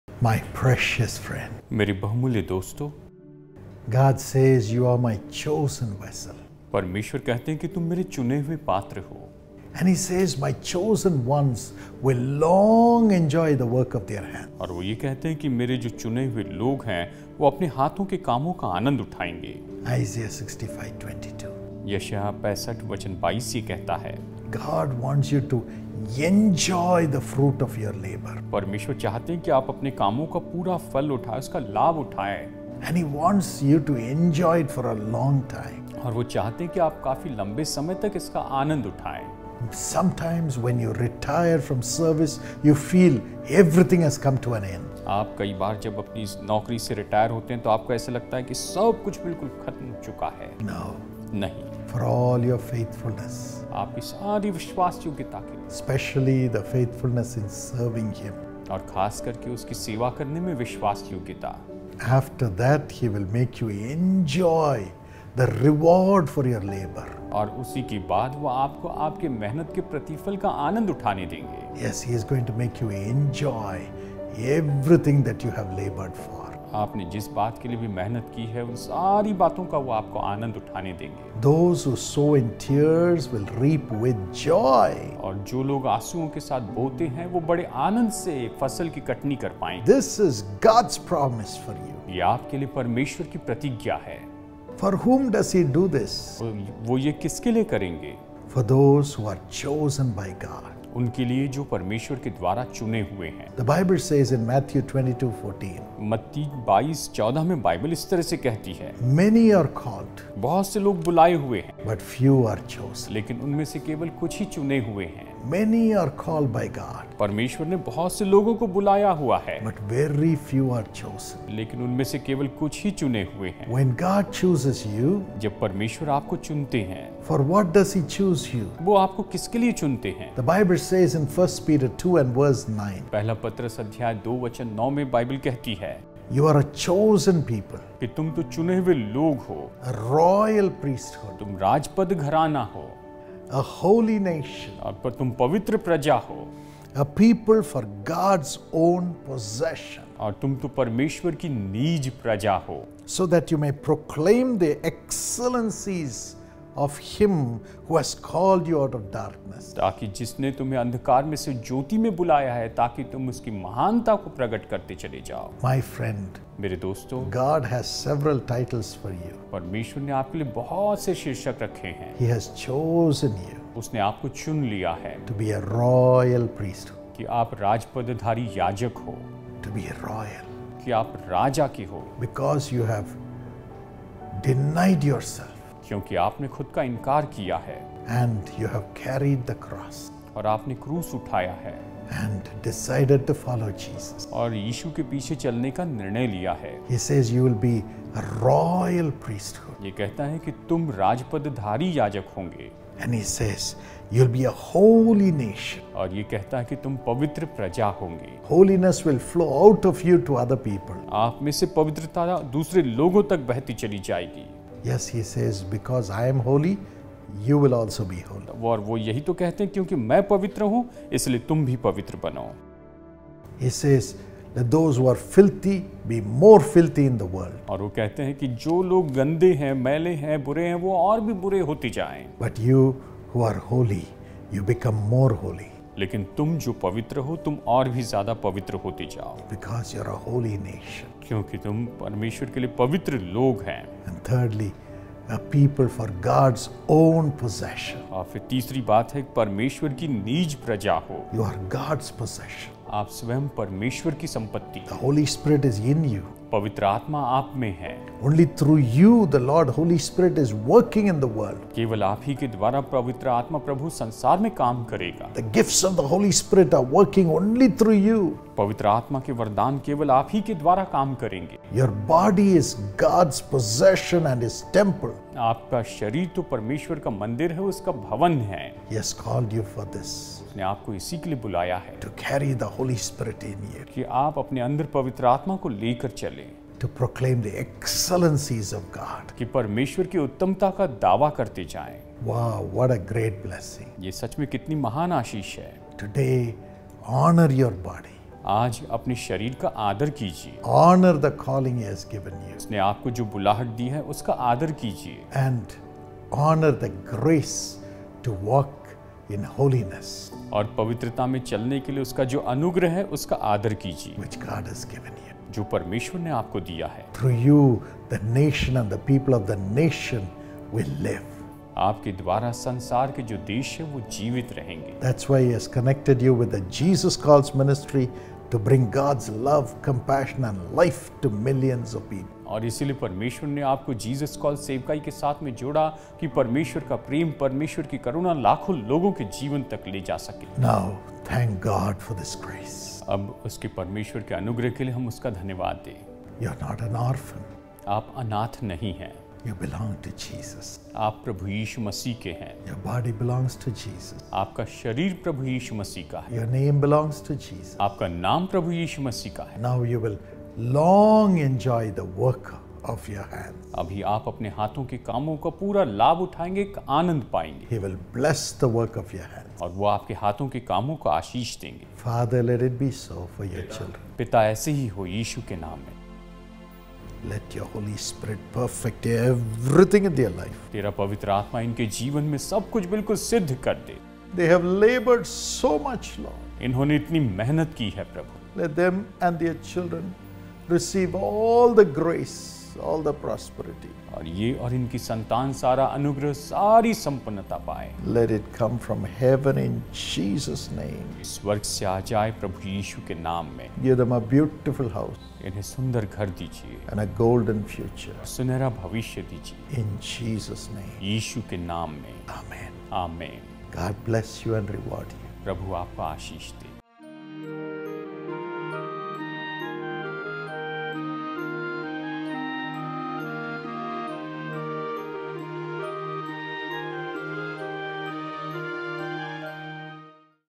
What Will Be the Reward For Your Work? | Dr. Paul Dhinakaran | Today's Blessing
You belong to Jesus, and so does your body and name. As you continue to work diligently, God will bless your efforts and make you prosper. Tune in to this blessing by watching today's promise.